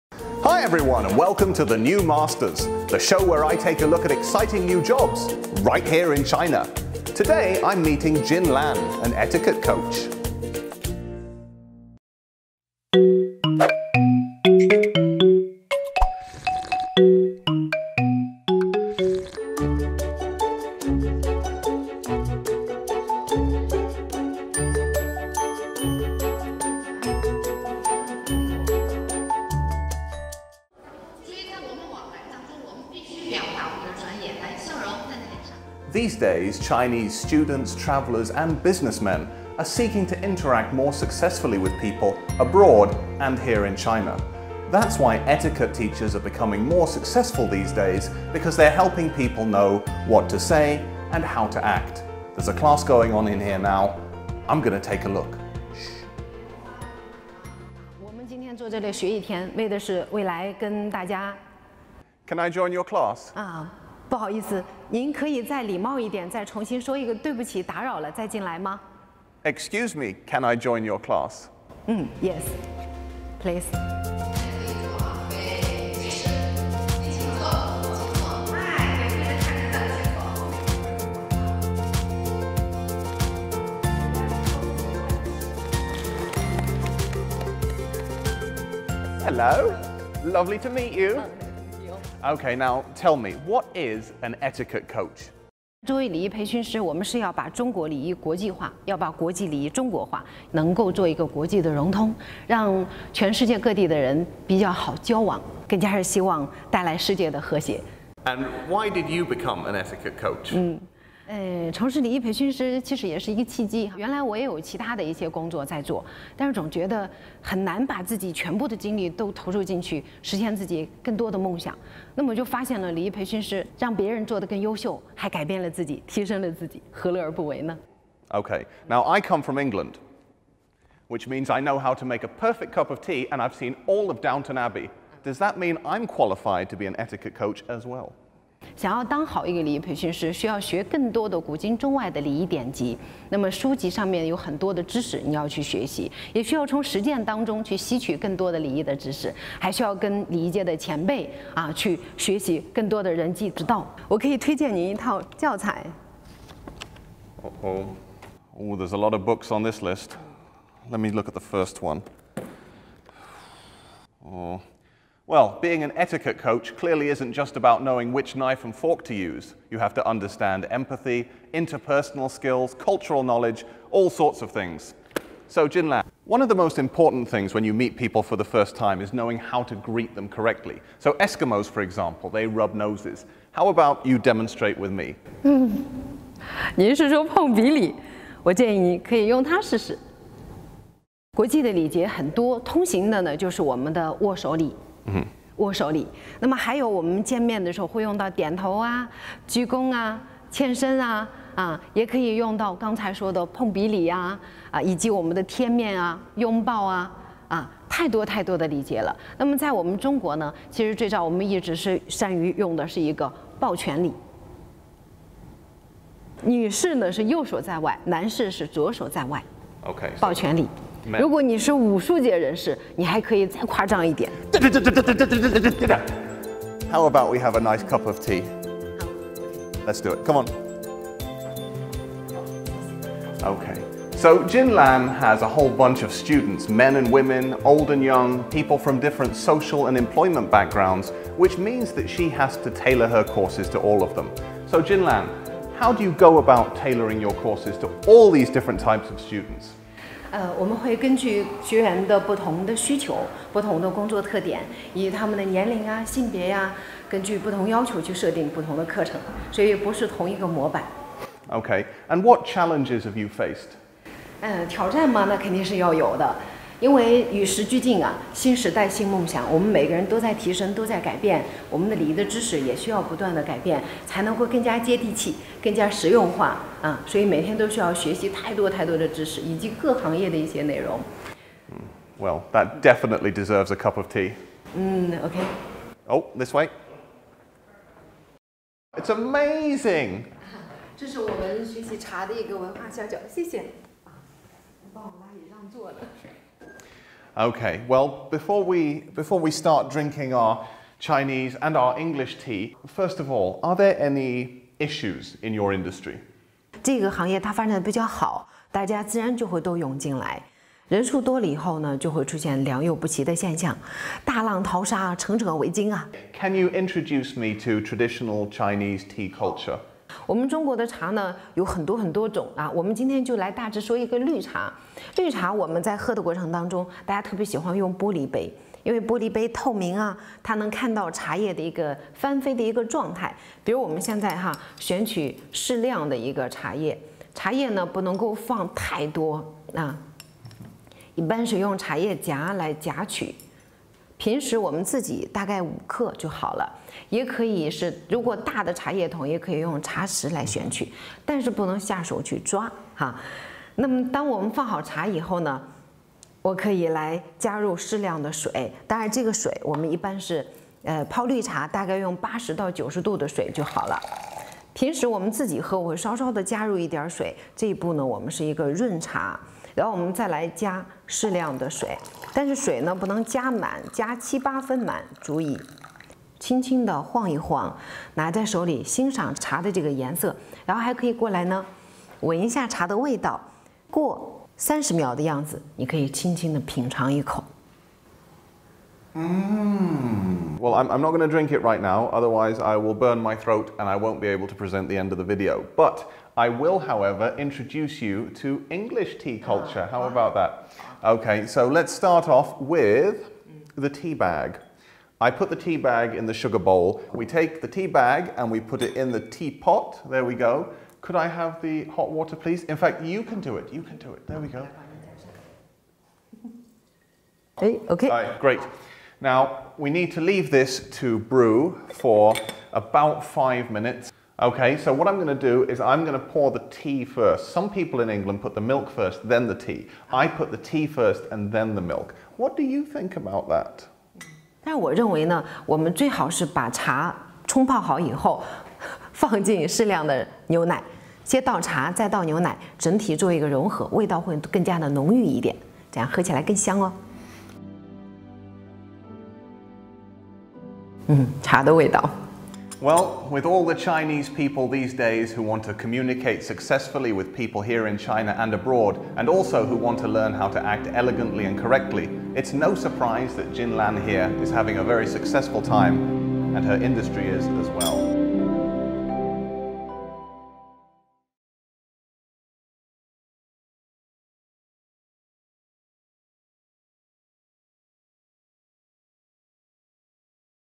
Etiquette Coach
Chinese students, travellers, and businessmen are keen to interact more successfully with people abroad and in China. Beijing-based etiquette coach Jin Lan helps to make that possible. With a strong focus on cultural knowledge, social empathy, and interpersonal communication, she designs courses for Chinese learners of all ages and backgrounds.